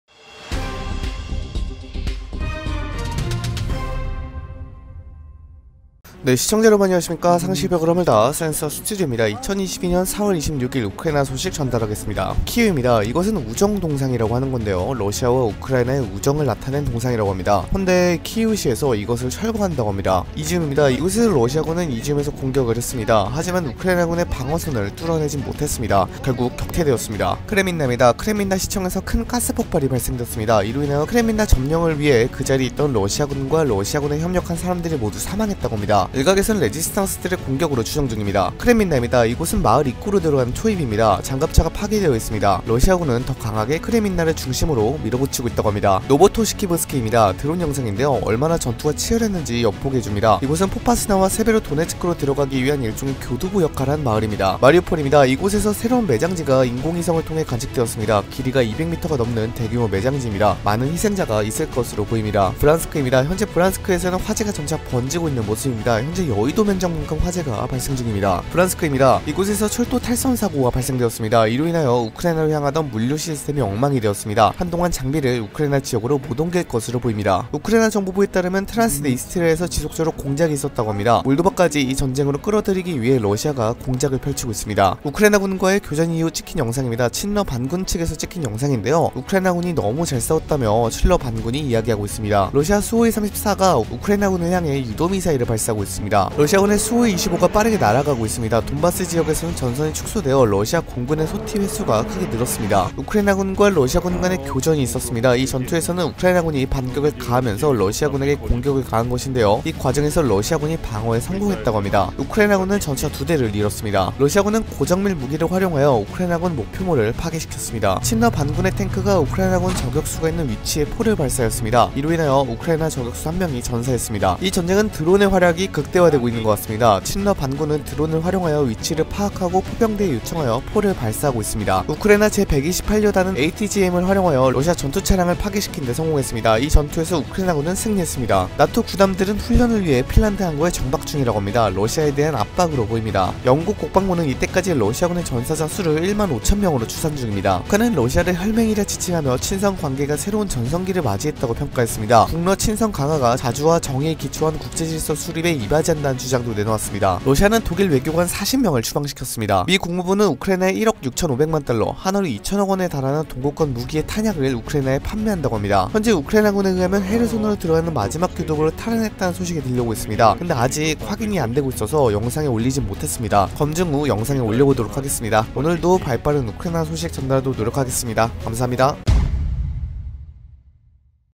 [속보] 러시아, 몰도바도 전쟁 끌어들이려고 해_우크라이나 긴급속보200_[센서 스튜디오]
키이우입니다. 이것은 우정 동상이라고 하는건데요. 러시아와 우크라이나의 우정을 나타낸 동상이라고 합니다. 헌데, 키이우시에서 이것을 철거한다고 합니다 - NEXTA

이지움입니다. 이곳에서 러시아군은 이지움에서 공격을 했습니다. 하지만 우크라이나군의 방어선을 뚫어내진 못했습니다. 결국 격퇴되었습니다. - 현지 제보(텔레그렘)

크레민나입니다. 크레민나 시청에서 큰 가스 폭발이 발생되었습니다. 이로 인하여 크레민나 점령을 위해 그 자리에 있던 러시아군과 러시아군에 협력한 사람들이 모두 사망했다고 합니다. 일각에서는 레지스탕스들의 공격으로 추정중입니다 - 키이우 인디펜던트

크레민나입니다. 이곳은 마을 입구로 들어가는 초입입니다. 장갑차가 파괴되어 있습니다. 러시아군은 더 강하게 크레민나를 중심으로 밀어붙히고 있다고 합니다  - 현지 제보(텔레그렘)

노보토시키프스케입니다. 드론 영상인데요. 얼마나 전투가 치열했는지 엿보게 해줍니다. 이곳은 포파스나와 세베로도네츠크로 들어가기 위한 일종의 교두보 역할을 한 마을입니다 - 현지 제보(텔레그렘)

마리우폴입니다. 이곳에서 새로운 매장지가 인공위성을 통해 관측되었습니다. 길이가 200m가 넘는 대규모 매장지입니다. 많은 희생자가 있을 것으로 보입니다 - NEXTA

브란스크입니다. 현재 브란스크에서는 화재가 점차 번지고 있는 모습입니다. 현재 여의도 면적만큼 화재가 발생중입니다 - NEXTA

브란스크입니다. 이곳에서 철도 탈선 사고가 발생되었습니다. 이로 인하여 우크라이나로 향하던 물류 시스템이 엉망이 되었습니다. 한동안 장비를 우크라이나 지역으로 못옮길 것으로 보입니다 - 현지 제보(텔레그렘)

우크라이나 정보부에 따르면 트란스니스트리아에서 지속적으로 공작이 있다고 합니다. 몰도바까지 이 전쟁으로 끌어들이기 위해 러시아가 공작을 펼치고 있습니다 - 키이우 인디펜던트

우크라이나군과의 교전 이후 찍힌 영상입니다. 친러반군측에서 찍힌 영상인데요. 우크라이나군이 너무 잘싸웠다며 친러반군이 이야기하고 있습니다. - 현지 제보(텔레그렘)

러시아 Su-34가 우크라이나군을 향해 유도 미사일을 발사하고 있는 모습입니다. - 현지 제보(텔레그렘)

러시아군의 Su-25가 빠르게 날아가고 있습니다. 돈바스 지역에서는 전선이 축소되어 러시아 공군의 소티 횟수가 크게 늘었습니다. - 현지 제보(텔레그렘)

우크라이나군과 러시아군간의 교전이 있었습니다. 이 전투에서는 우크라이나군이 반격을 가하면서 러시아군에게 공격을 가한 것인데요. 이 과정에서 러시아군이 방어에 성공했다고 합니다. 우크라이나군은 전차 2대를 잃었습니다 - 현지 제보(텔레그렘)

러시아군은 고정밀 무기를 활용하여 우크라이나군 목표물을 파괴시켰습니다. - 현지 제보(텔레그렘)

친러반군의 탱크가 우크라이나군 저격수가 있는 위치에 포를 발사하였습니다. 이로 인하여 우크라이나 저격수 1명이 전사했습니다. - 현지 제보(텔레그렘)

이 전쟁은 드론의 활약이 극대화되고 있는 것 같습니다. 친러반군은 드론을 활용하여 위치를 파악하고 포병대에 요청하여 포를 발사하고 있습니다. - 현지 제보(텔레그렘)

우크라이나 제128여단은 ATGM을 활용하여 러시아 전투차량을 파괴시키는데 성공했습니다. 이 전투에서 우크라이나군은 승리했습니다. - 현지 제보(텔레그렘)

NATO 군함들은 훈련을 위해 핀란드 항구에 정박중이라고 합니다. 러시아에 대한 압박으로 보입니다 - israelhayom

영국 국방부는 이때까지 러시아군의 전사자 수를 1만 5000명으로 추산중입니다 - 데보라 헤인즈

북한은 러시아를 혈맹이라 지칭하며 친선관계가 새로운 전성기를 맞이했다고 평가했습니다. 북러 친선 강화가 자주와 정의에 기초한 국제질서 수립에 이바지한다는 주장도 내놓았습니다 - 뉴시스

러시아는 독일 외교관 40명을 추방시켰습니다 - CNN

미 국무부는 우크라이나에 1억 6500만 달러. 한화로 2000억원에 달하는 동구권 무기의 탄약을 우크라이나에 판매한다고 합니다 - 프란츠 스테판 가디